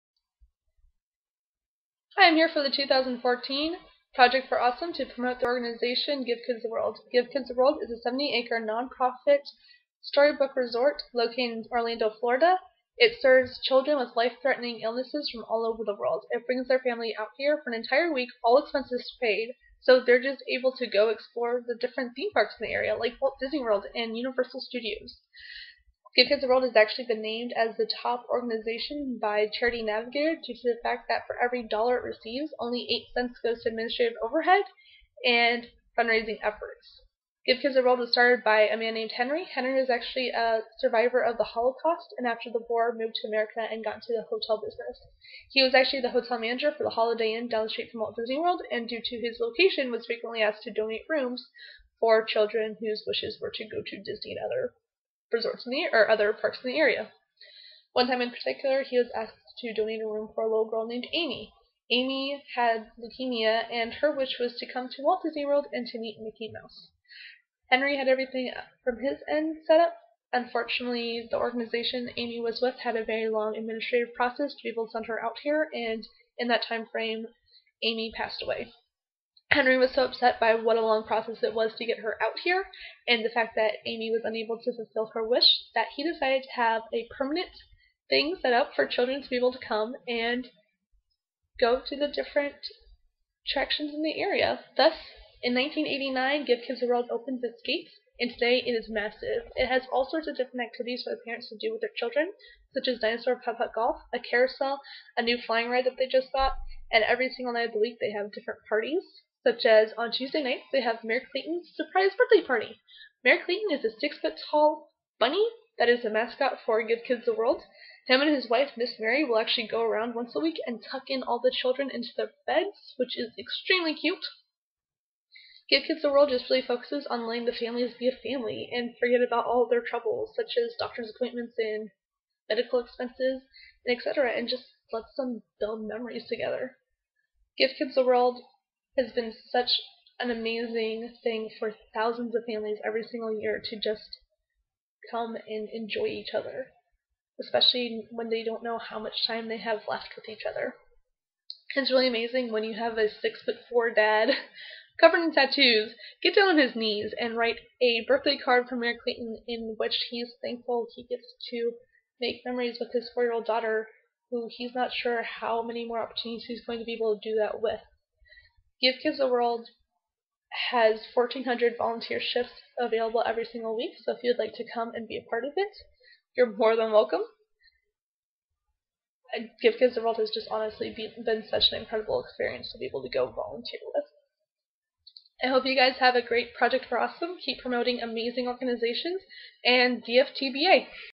Project For Awesome 2014 Give Kids the World
First time ever submitting a video in which I talk. But I am here to promote the amazing organization Give Kids the World. It's a 70 acre nonprofit storybook resort for families with children who have life threatening illnesses.

DFTBA!